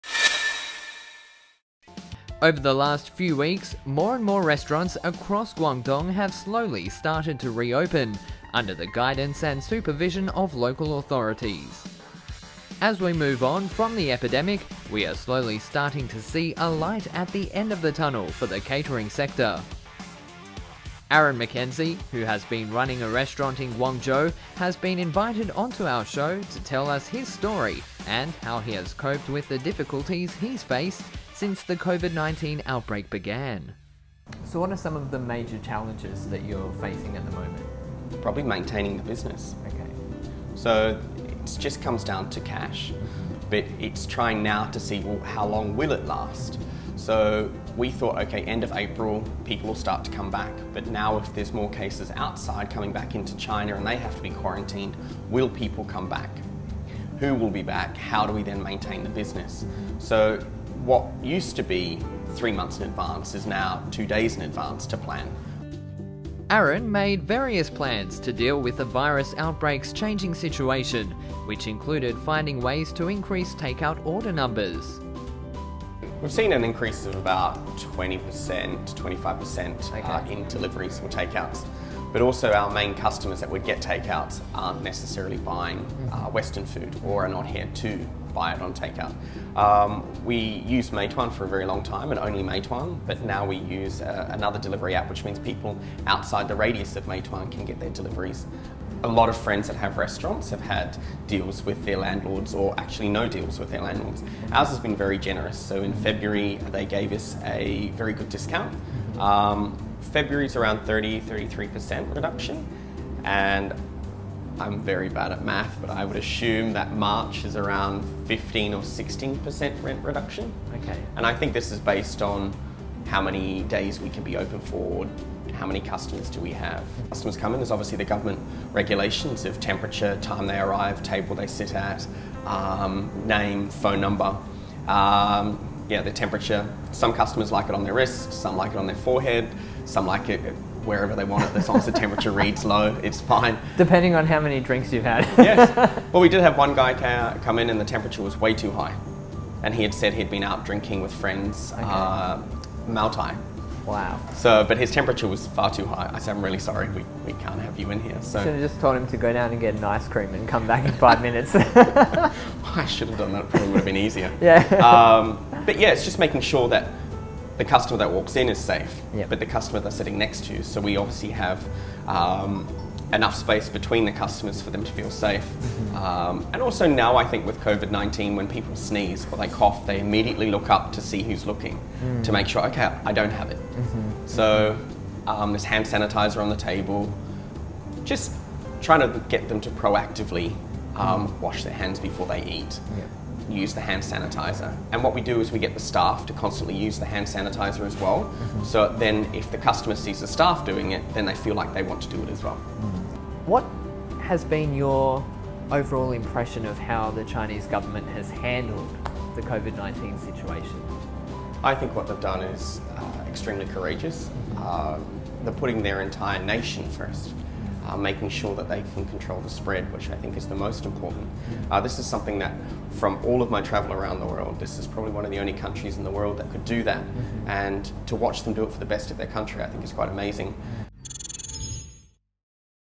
广州餐饮复工进行时 Refiring the works-GZ's F&B business rebounds
来自新西兰的阿伦在广州开了一家西餐厅。六年来他一直在广州经营并以广州为家。疫情当中，他的员工都团结一致，想各种办法增加营业额； 他的房东给他减租帮助他缓解现金难题；甚至曾经的竞争对手都变成合作伙伴，一起渡过难关。他相信疫情终将过去，广州餐饮业的春天即将到来。
Aaron from New Zealand opened a restaurant in Guangzhou, where he has stayed for six years and treated as home. During the epidemic, his employees have been united to find ways to increase turnover. His landlord gave him rent reduction to help him ease the cash problem. Even his competitors became partners to tide over the difficulties together. He believed that the epidemic would be over and Guangzhou’s F&B business will rebound.